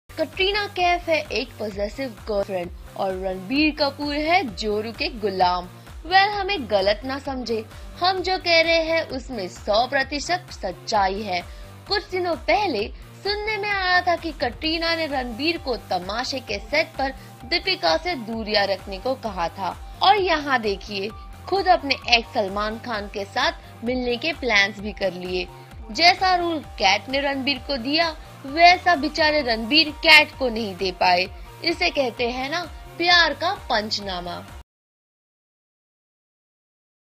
Katrina acts BOSSY with Ranbir !!!
Katrina Kaif acts bossy with Ranbir & dominates over him, findout more in this Video.